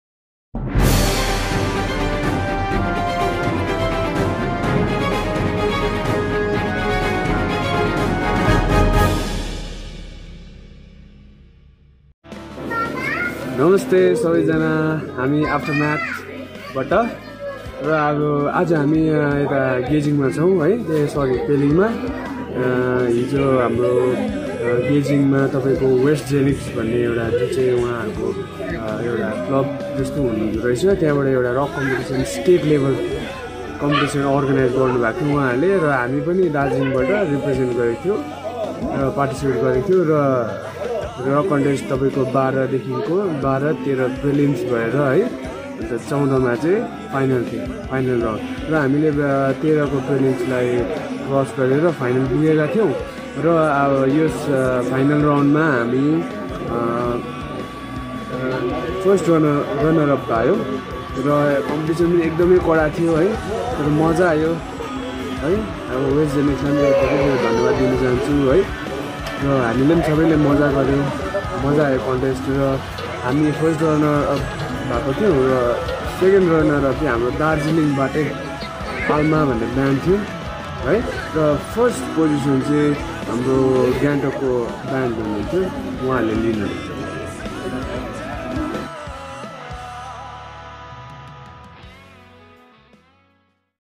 Darjeeling News | पहिलो उपविजेता बनेर खुशी व्यक्त गर्दै ब्याण्ड "Aftermath"
State level Rock contest held at Gyalshing (Geyzing) west sikkim. Organized by West Genics
Held on 12th 13th 14th

Band - THE AFTERMATH
position -1st runners up